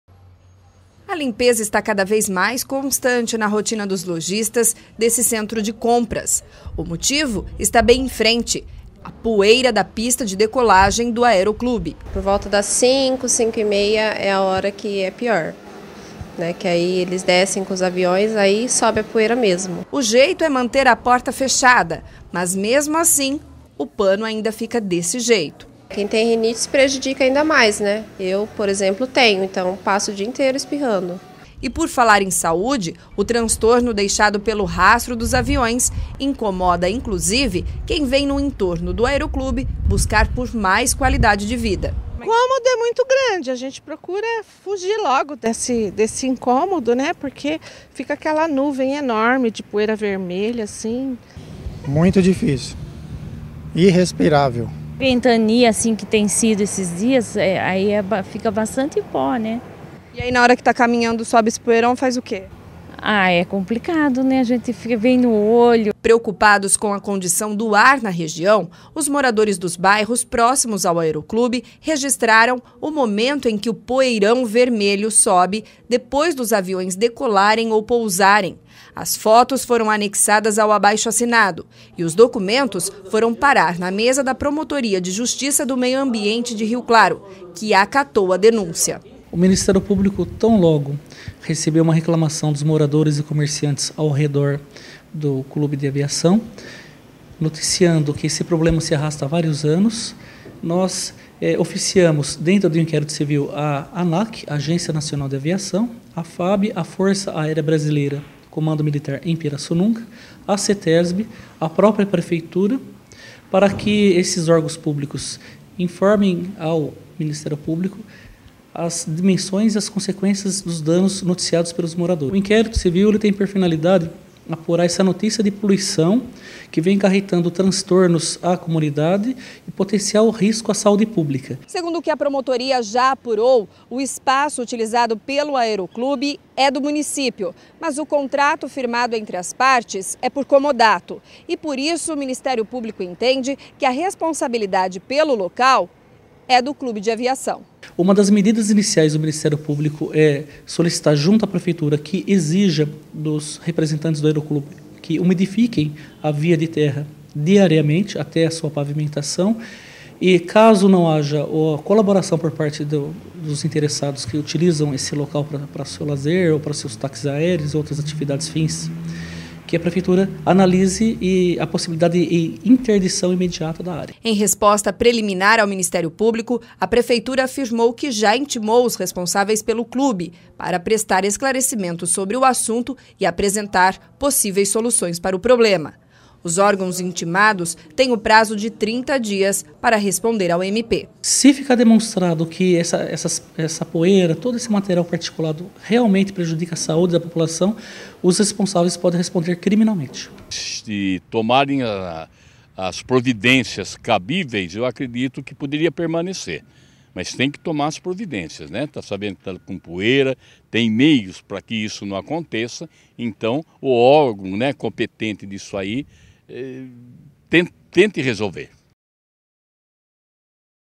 Justiça instaura inquérito para apurar poluição do aeroclube em Rio Claro
Problemas com pousos e decolagens em pista de terra foi apontado pelo Ministério Público e acatado pela justiça. Inquérito civil pede, entre outras coisas, que a prefeitura avalie reduzir pousos e decolagens até a pavimentação do trecho.

Em nota, a prefeitura de Rio Claro disse que foi comunicada da abertura do inquérito civil em desfavor do aeroclube para que adote as medidas administrativas de âmbito municipal. A prefeitura tomará as medidas cabíveis.

O aeroclube não quis se pronunciar.